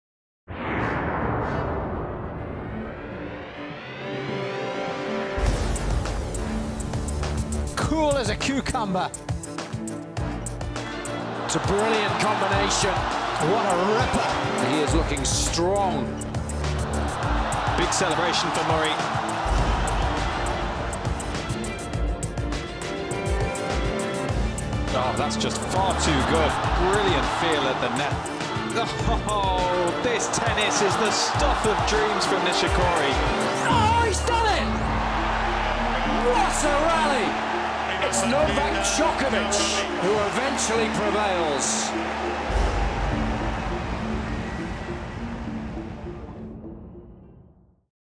Rome 2016 SF Preview
Andy Murray will face Lucas Pouille, while Novak Djokovic will take on Kei Nishikori in Rome on Saturday. Watch live tennis at tennistv.com. Photo: Getty Images